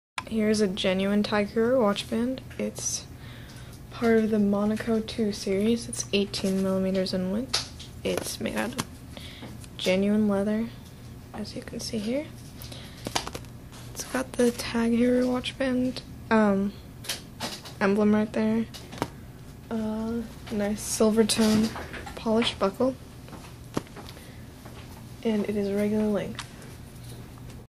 Tag Heuer Monaco II Black Leather 18mm Band
Tag Heuer, Monaco II Series, 18mm, Men's, Black, Genuine Leather Strap, Silver Tone Polished Buckle, Regular Length.

Model No. BC0787